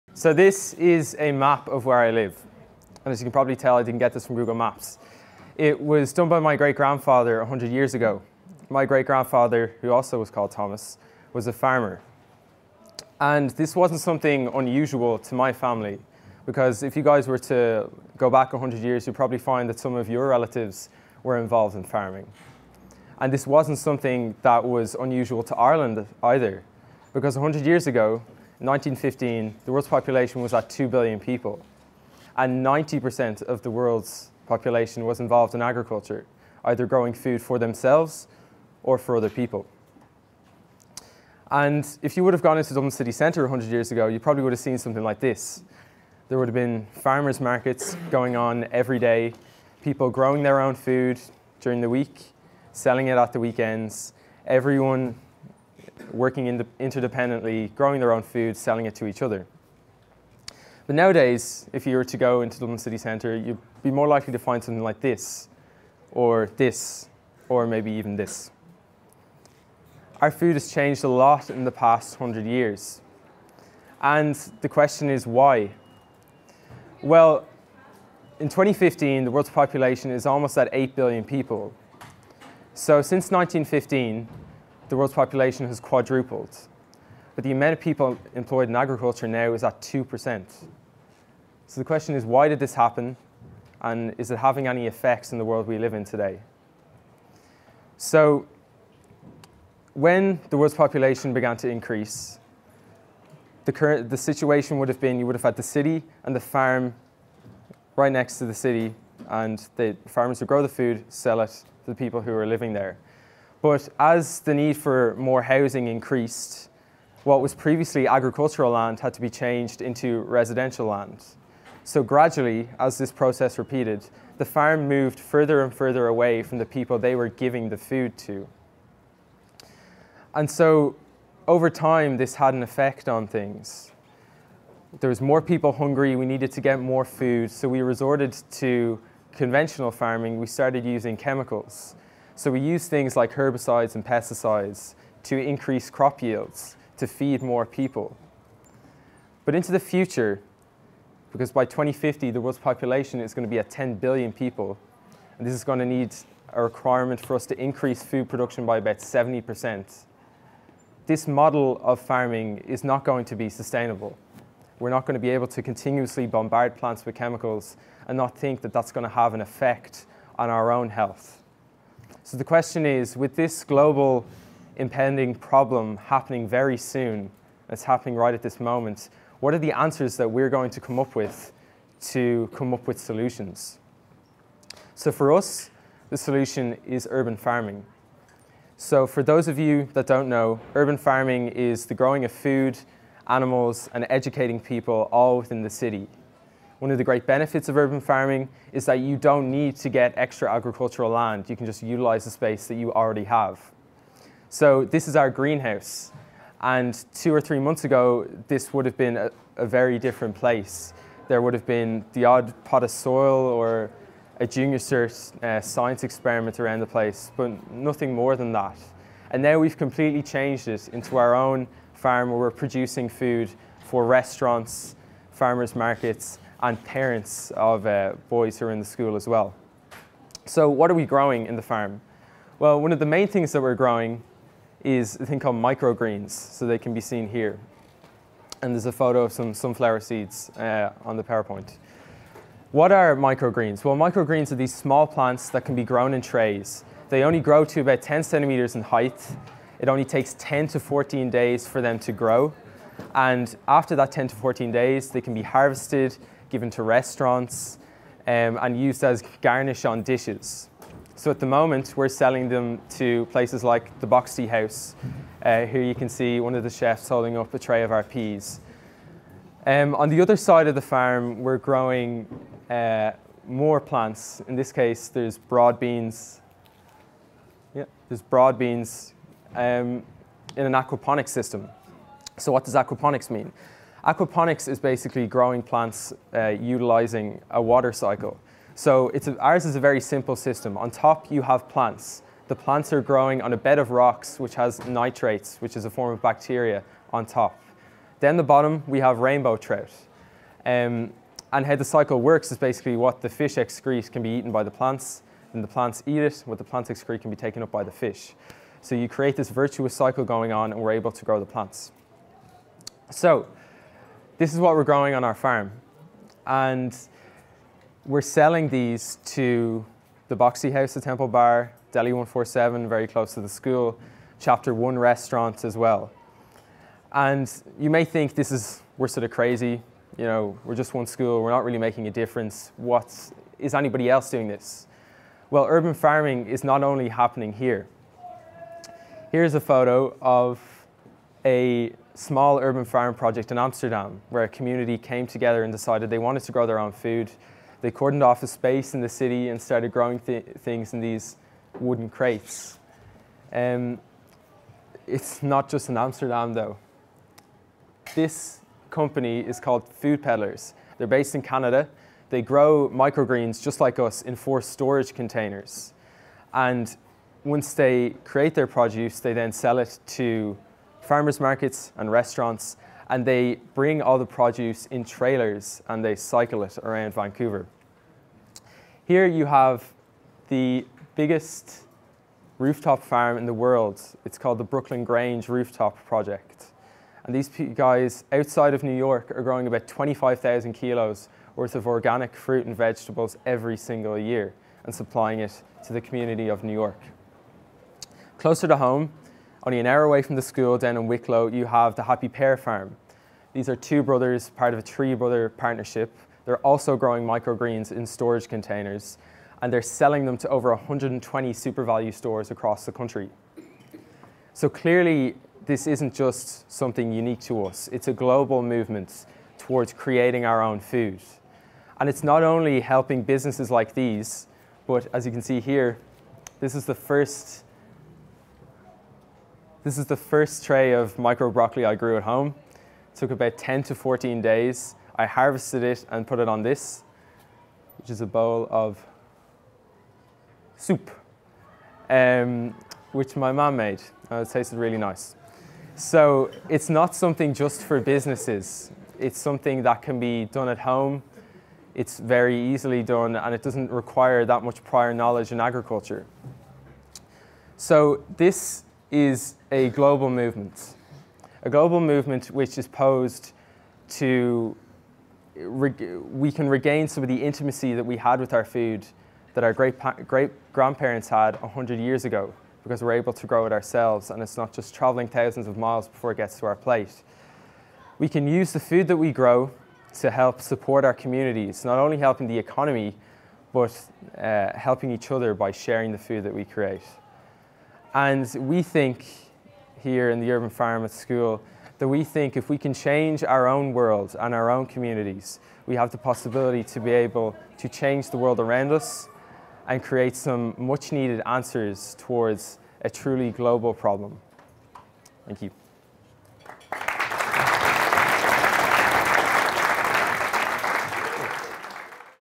The Future of Food - Belvedere Urban Farm
We are a Dublin based urban farming project set to change our world one seed at a time.

The school greenhouse was converted into our own GROWlab at the start of 2015.  The space is now used to grow microgreens such as wheatgrass, micro broccoli, radish, fenugreek, peas as well as herbs, fruits and vegetables via Aquaponics.  We sell this produce on to local restaurants and Farmer's markets.  This summer we are launching grow kits so students can grow their own food at home.  We have only just begun.

This video has been created to spread the word about the revolution that is Urban Farming.  We hope you want to join in.

We put up a blog post every weekend to update our Farming followers of what is going on in the GROWlab:

Video filmed by Alex Quinn of bold move productions, check out his awesome work